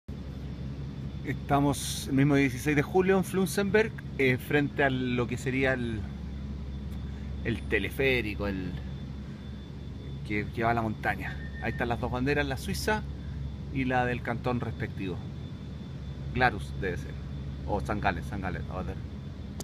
3 SACSG. " Suiza Alemana en Cantón St. Gallen: localidad Flumseeberg "
Este centro de Esquí en el norte de la comuna de Flums, dentro del norte del distrito de Sarganserland, que a la vez está en el sector sur del Cantón de St. Gallen, a la vez en sector a extremo nororiente de Suiza. Este lugar es perteneciente a la macrozona cultural y lingüísticamente denominada Suiza Alemana. Vista a las banderas de Suiza y del cantón, y a los montes del Walensee (Lago Walen). S 16 Jul 2016